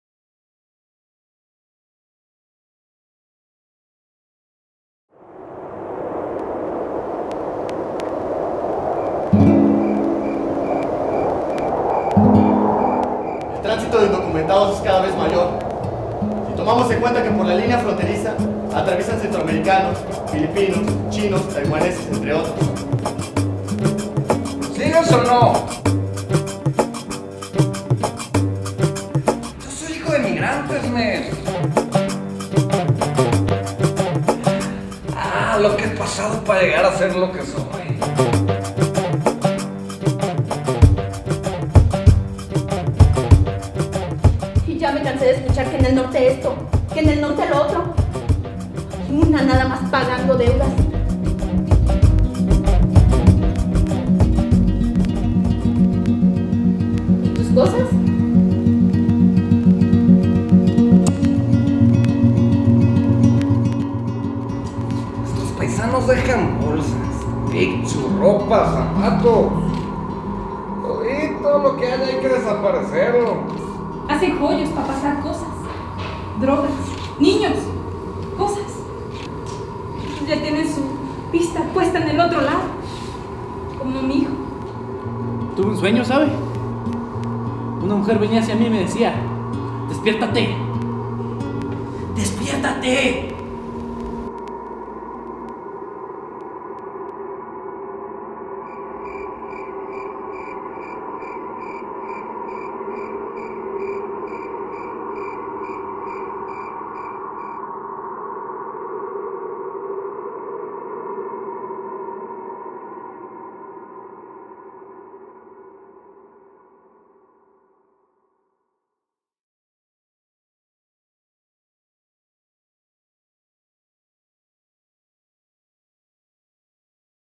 La Cubeta de los Cangrejos
Trailer promocional de la obra de teatro: "La Cubeta de los Cangrejos" de Juan Carlos Embriz, y montada por el taller de Teatro de Eduardo Villalpando.
Promotional trailer of the play: "La Cubeta de los Cangrejos" (The crab's bucket) by Juan Carlos Embriz and staged by the Eduardo Villalpando theater workshop.